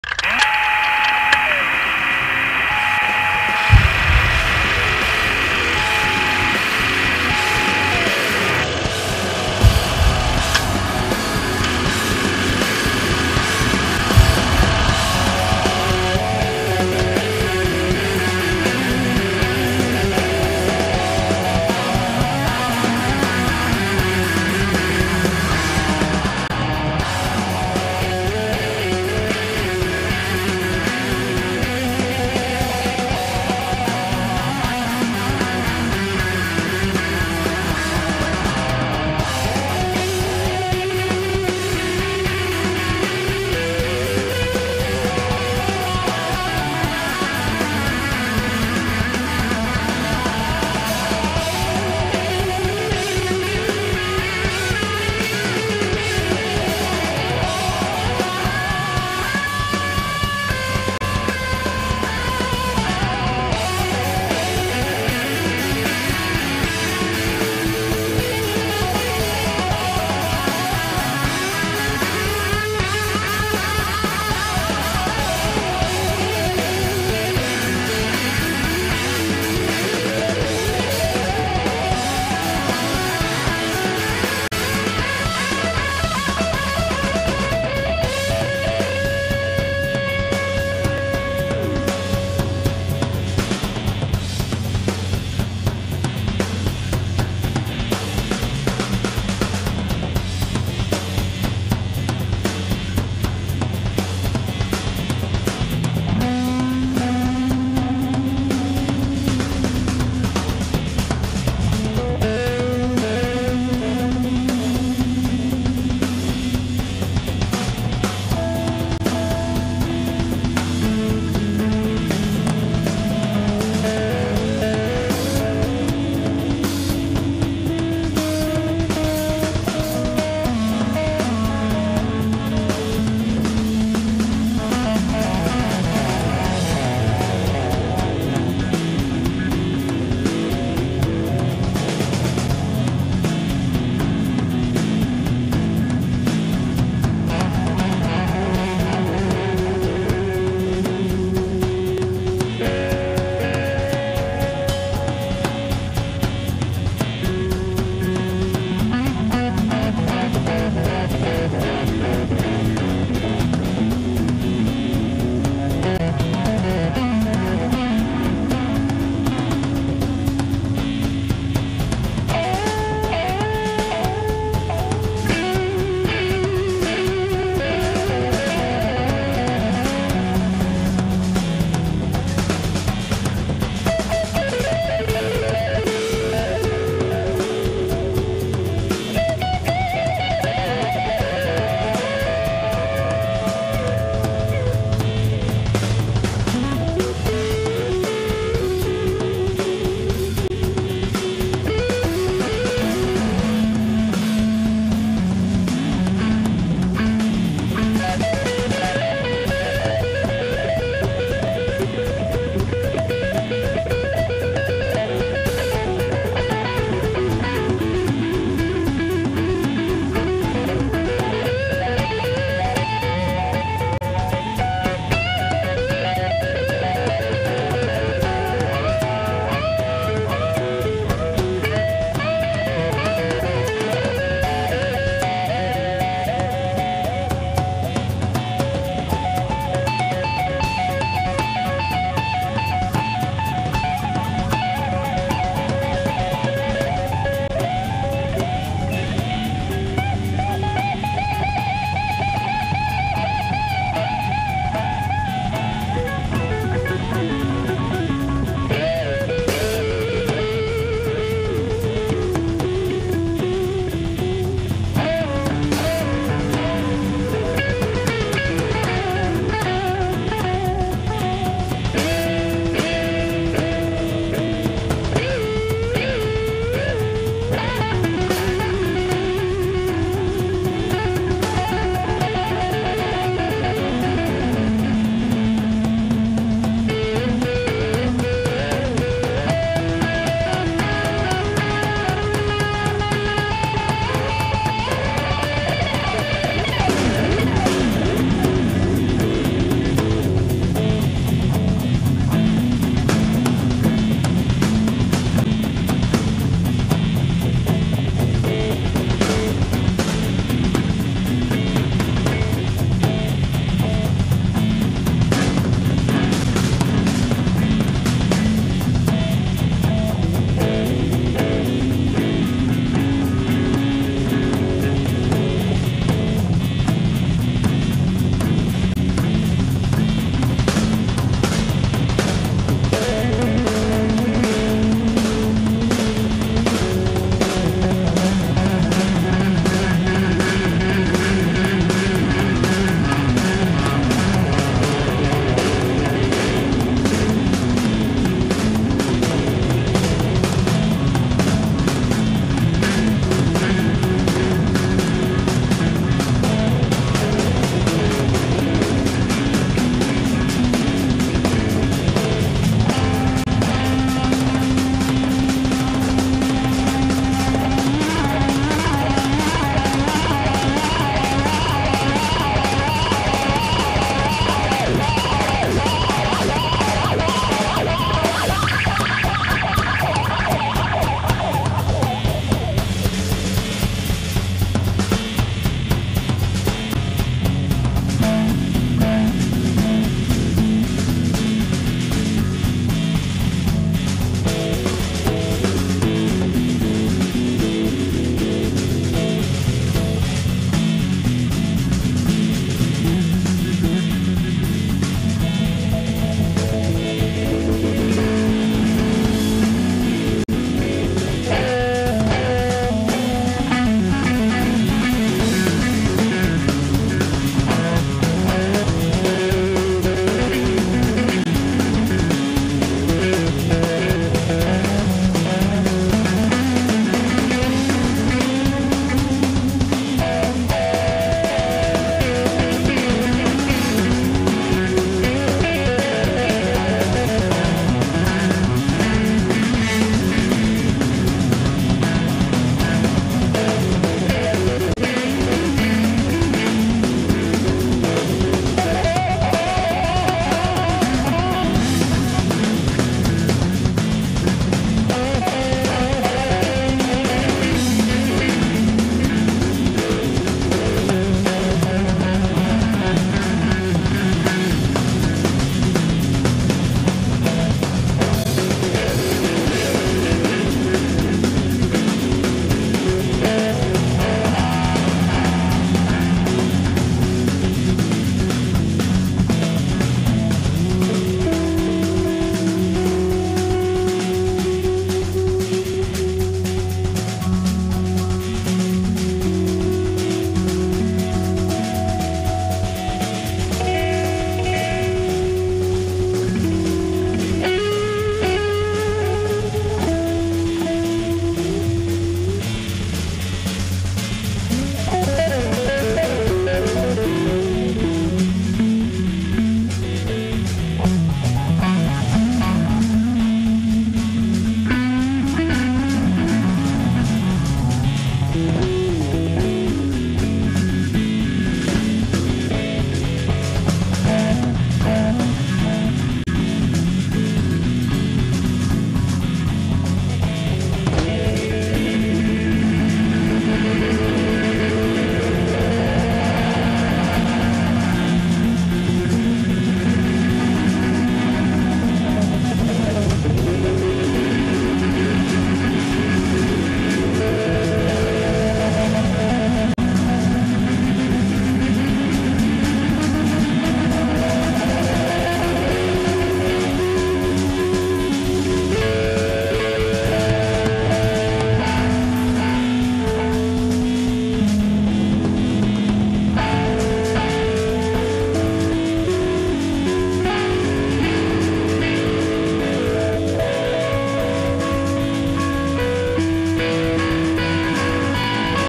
Earthless live at Supersonic - Part 1
Earthless live at Supersonic Festival 2009, Filmed and edited by HTF Media.